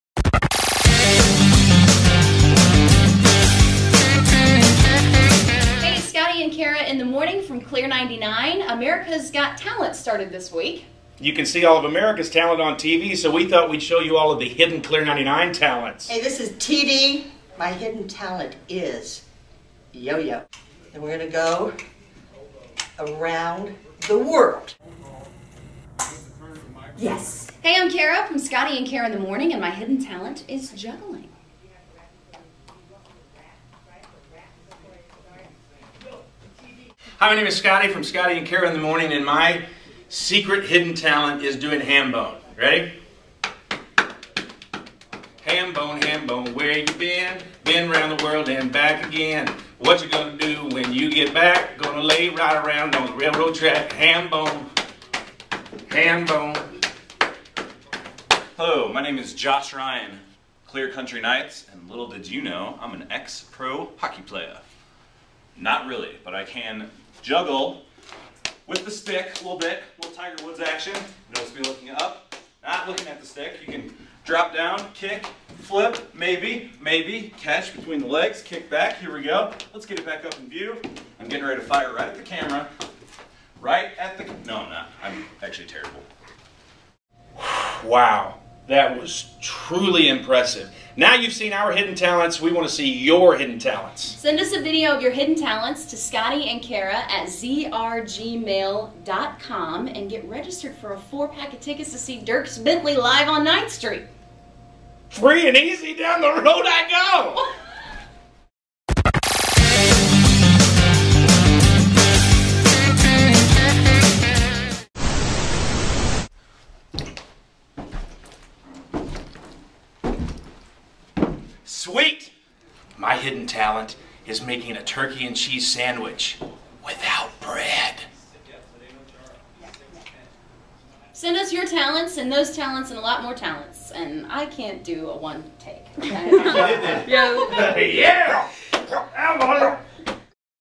Clear99 Hidden Talents
The DJs of Clear 99 have plenty of hidden talents...send a video of YOUR hidden talent to to win a 4-pack of tickets to see Dierks Bentley live on Ninth Street!